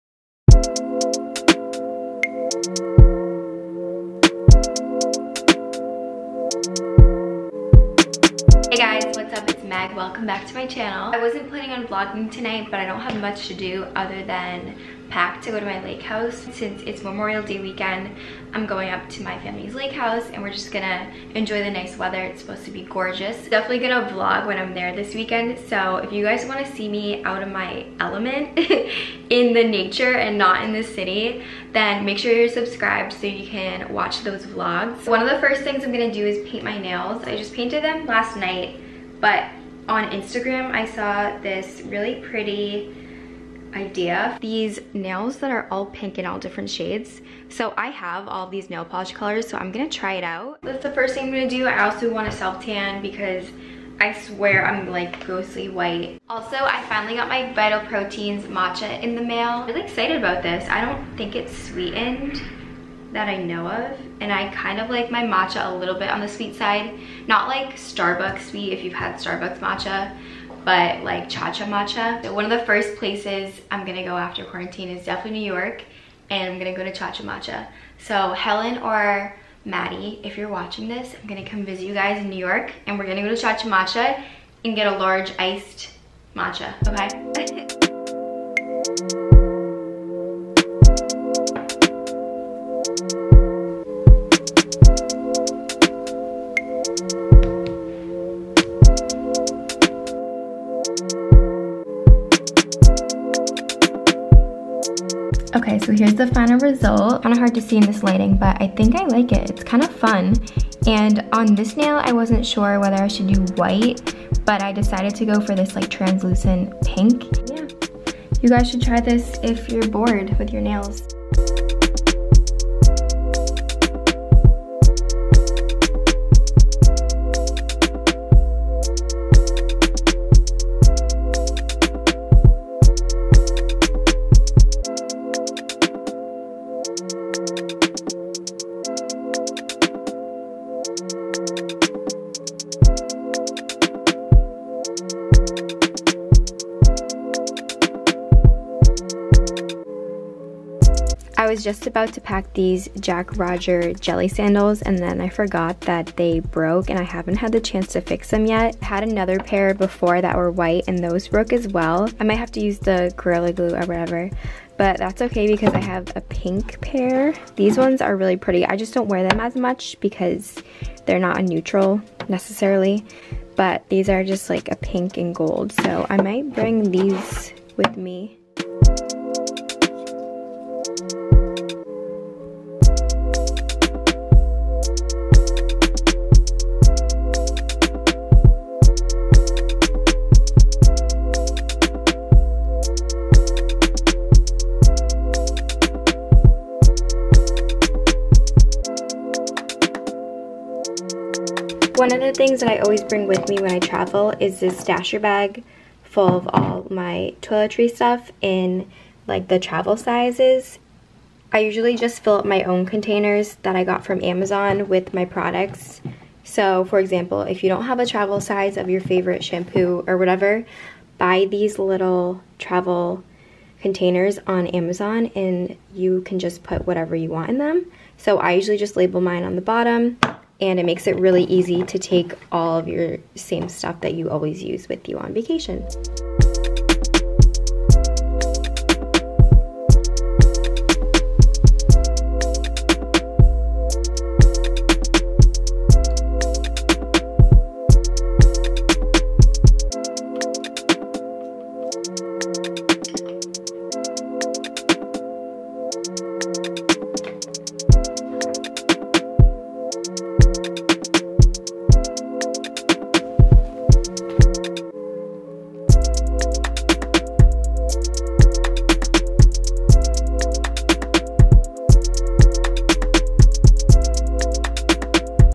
VLOG ~ pack with me! leaving the city + new nails
another mini vlog ~ longer vlogs will be coming soon!
hiii, pack with me to leave boston for a few days! let me know if you guys like the nails, I got lazy in the vlog while filming so I didn't finish the other hand hahaha

link for amazon travel jars!

xoxo,
meg